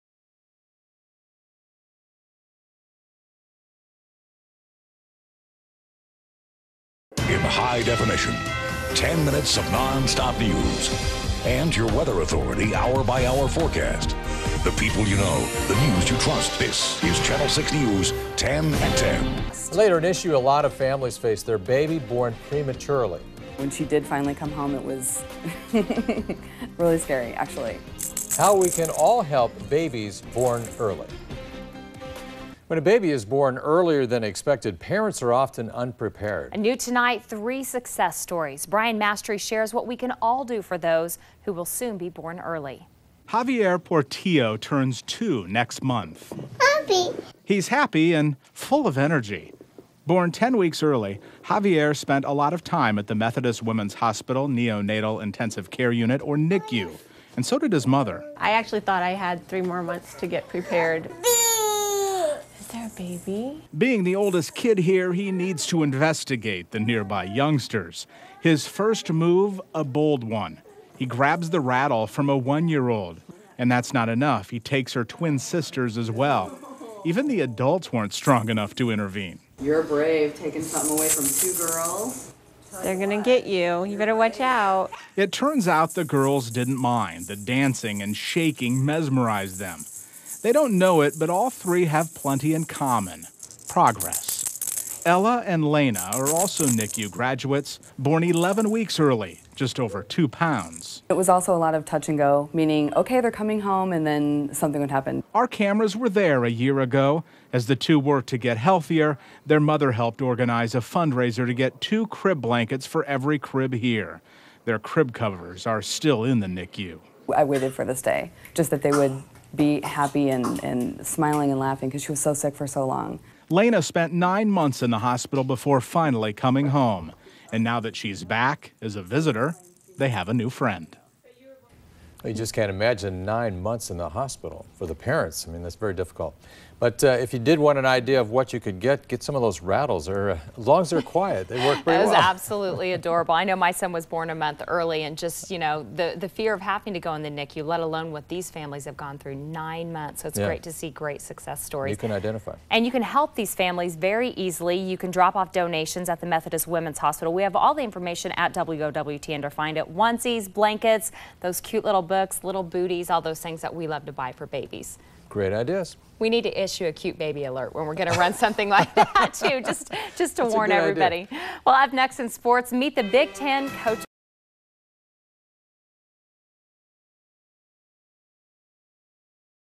Healthy, happy graduates of NICU
Ella and Lana Rohrbach and Javier Portillo are just three of many success stories for the Methodist Women's Hospital Neonatal Intensive Care Unit.